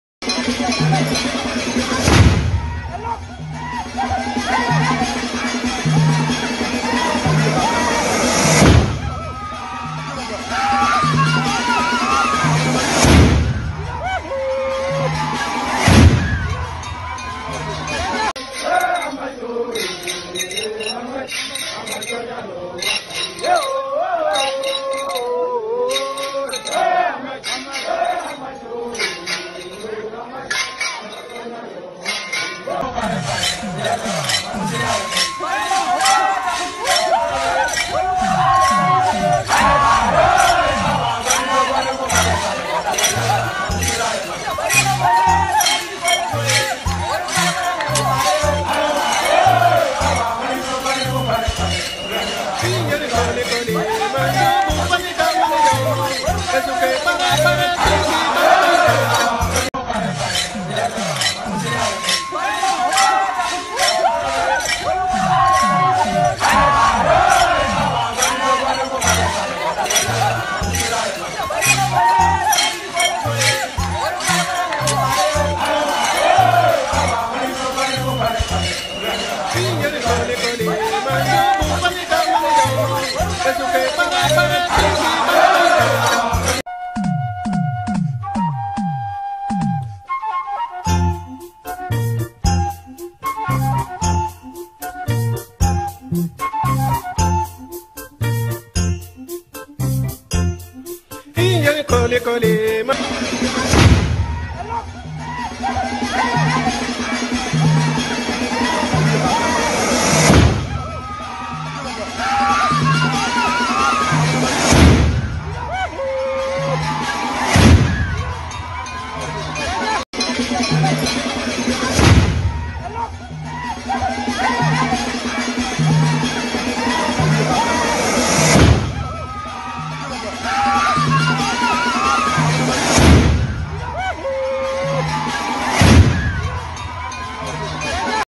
Jola Culture
Initiation ceremony 2022 in Balingore Casamace. It is known as "Bukut" in Jola Language.